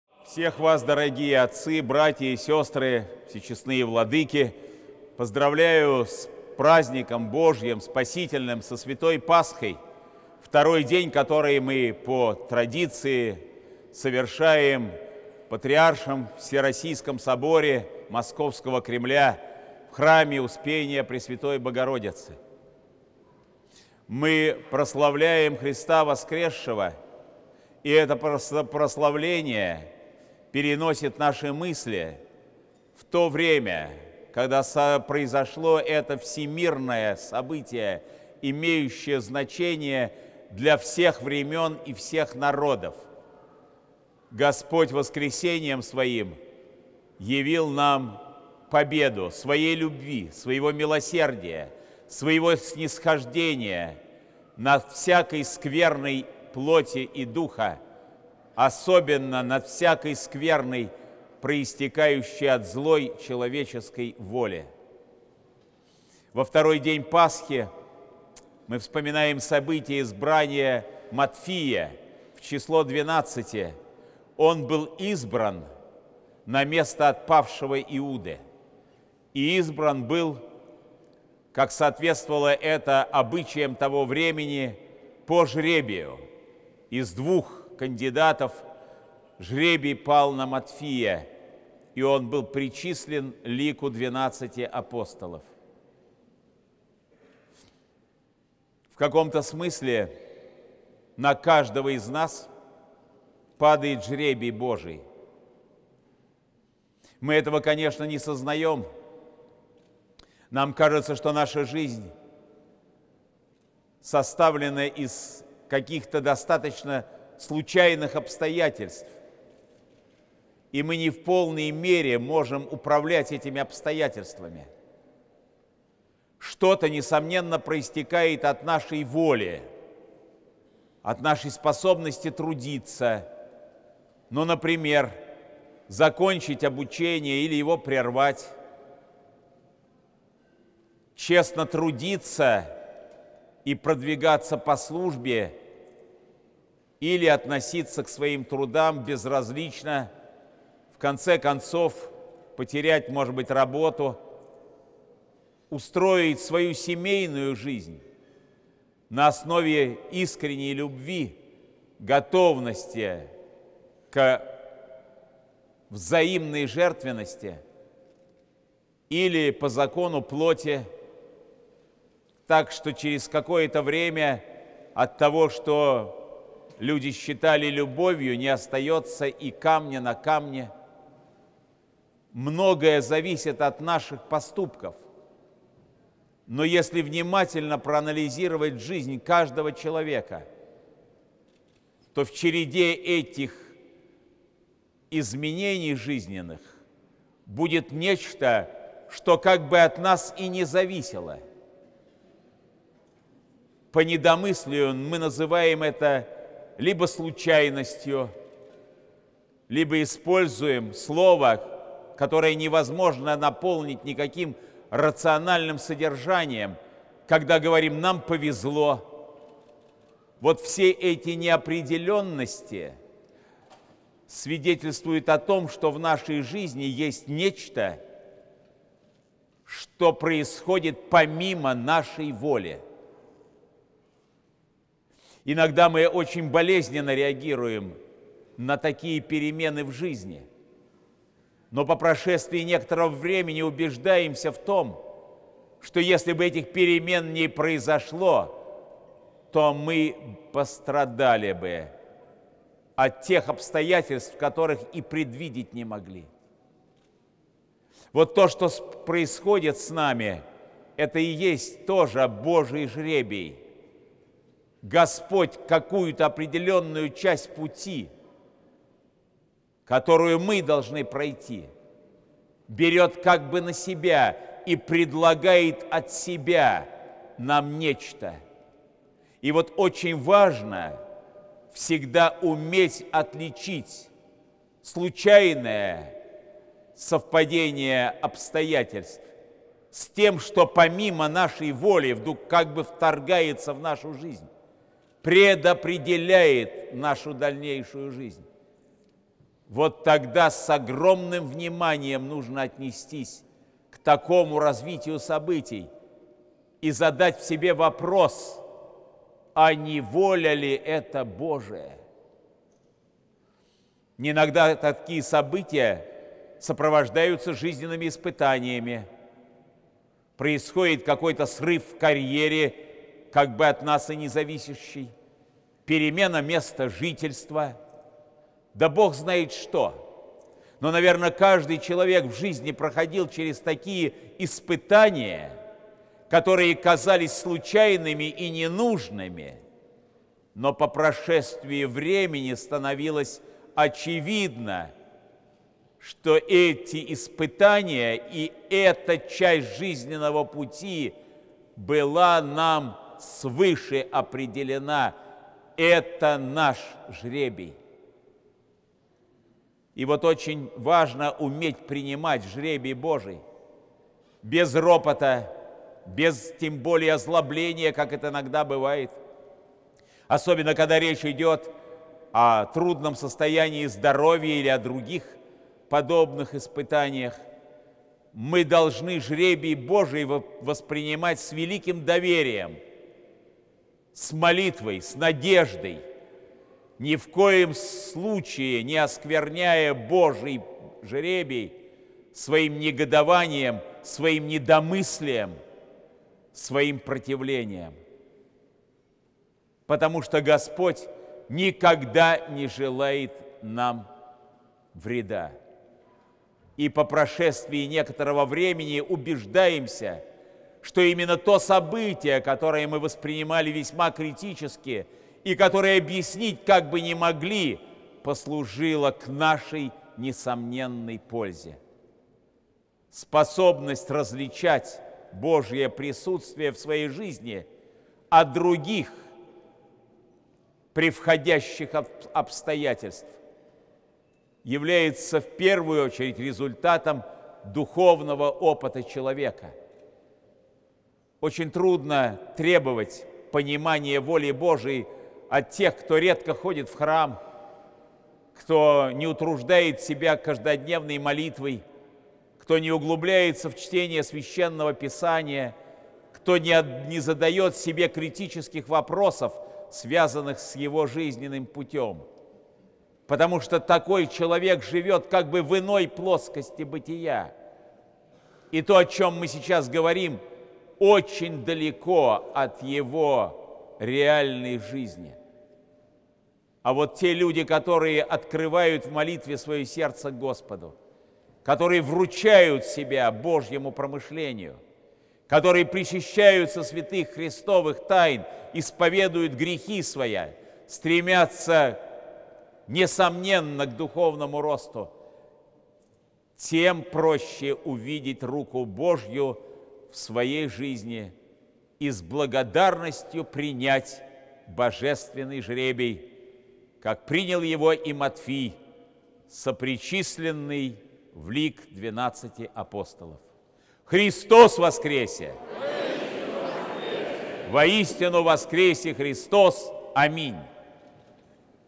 Проповедь Патриарха Кирилла в понедельник Светлой седмицы
17 апреля 2017 года, в понедельник Светлой седмицы, Святейший Патриарх Кирилл совершил Божественную литургию в Патриаршем Успенском соборе Московского Кремля.